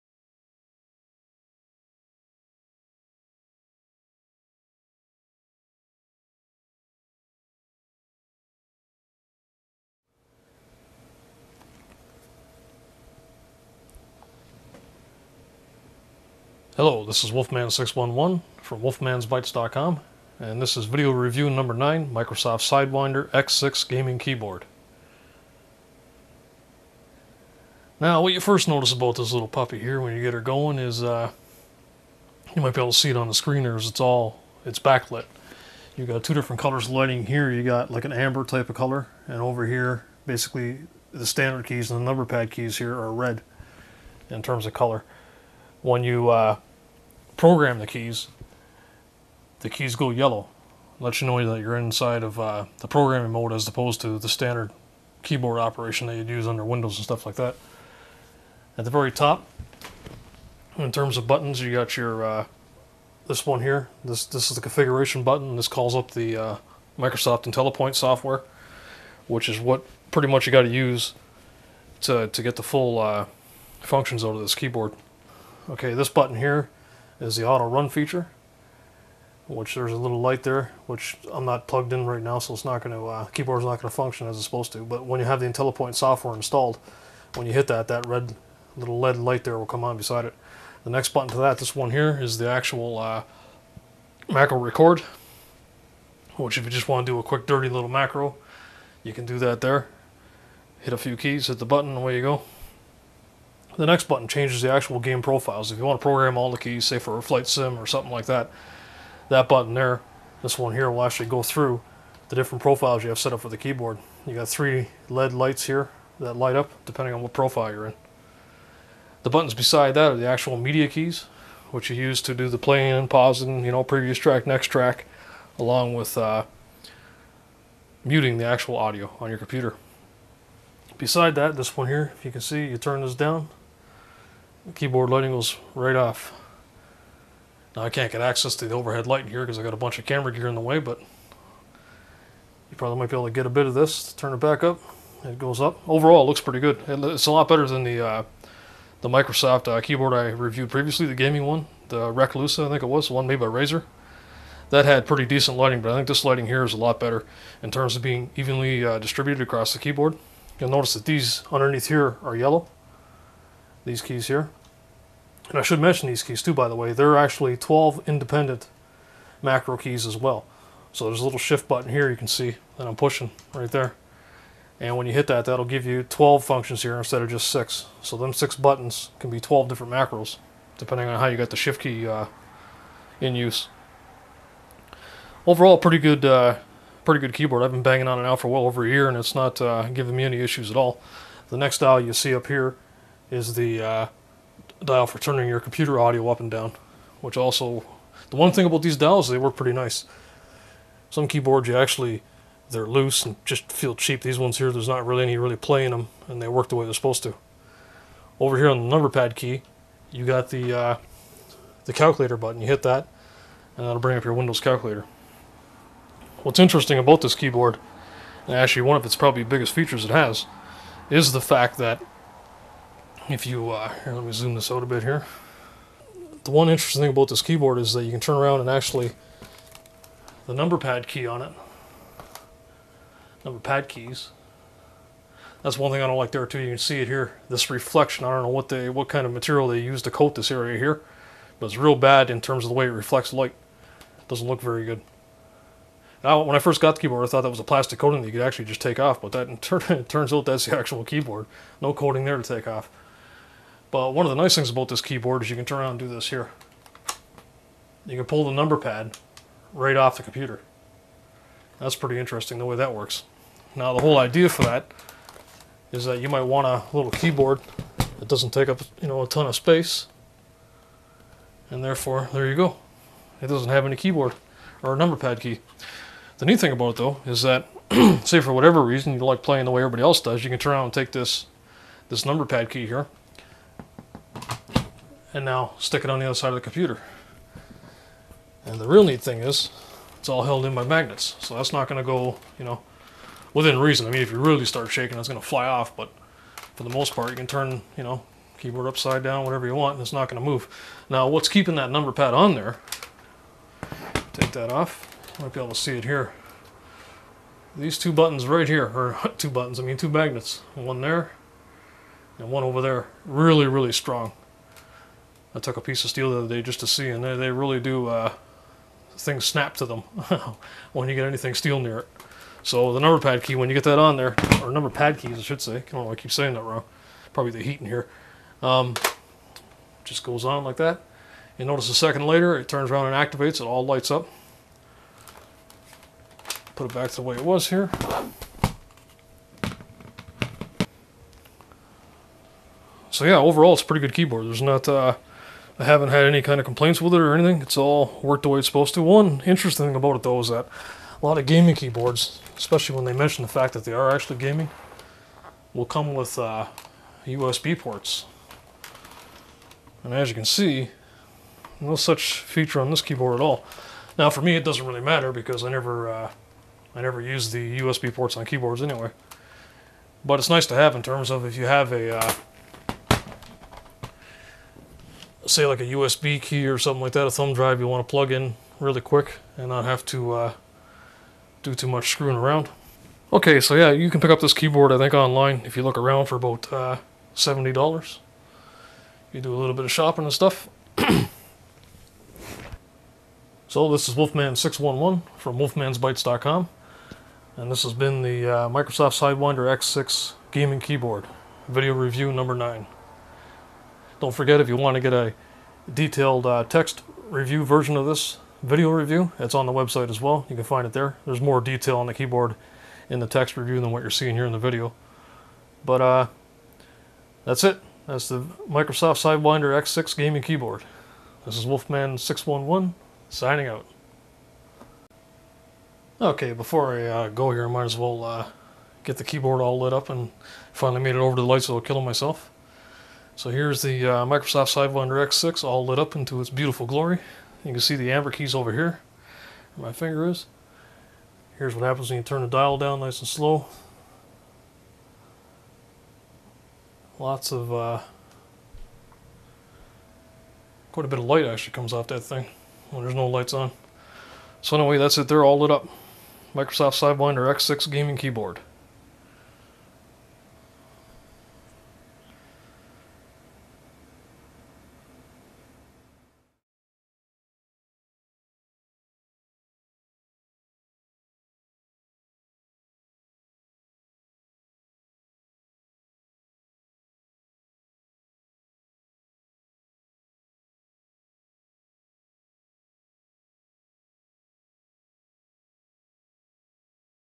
Microsoft Siderwinder X6 Gaming Keyboard.
Review of the Microsoft Sidewinder X6 Gaming Keyboard.